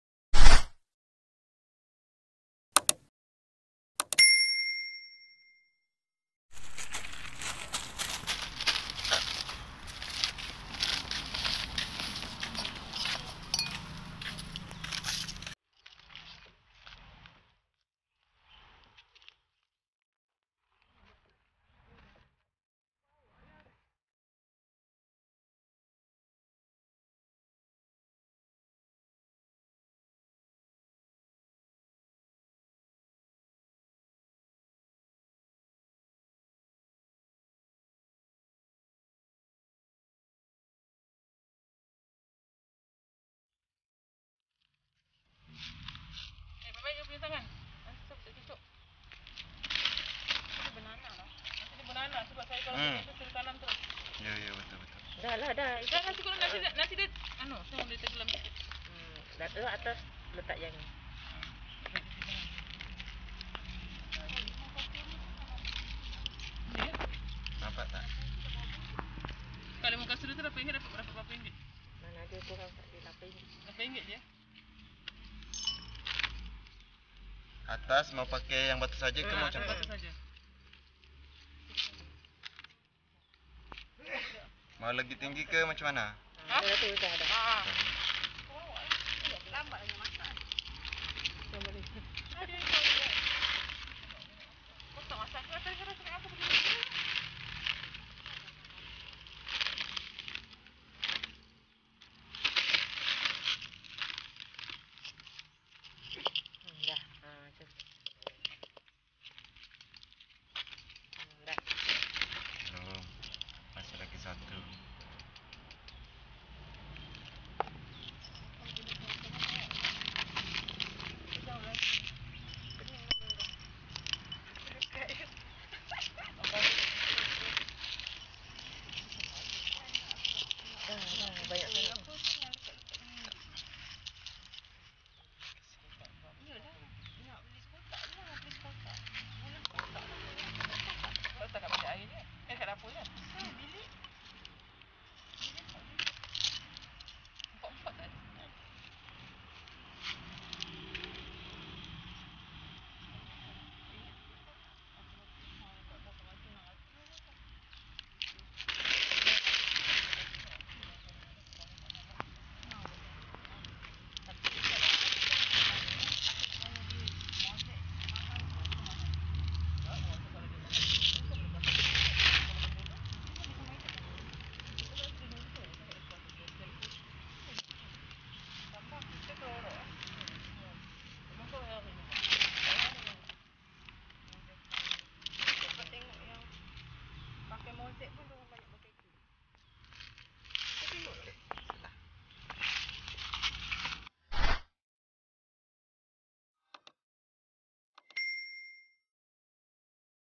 CARA REPOTTING KAKTU/CACTUS KE POT TERAKOTA.
Cara menanam kaktus.cara menanam sukulen.cara menanam cactus .cara menanam secullen.cara menjaga kaktus.

dhens lombok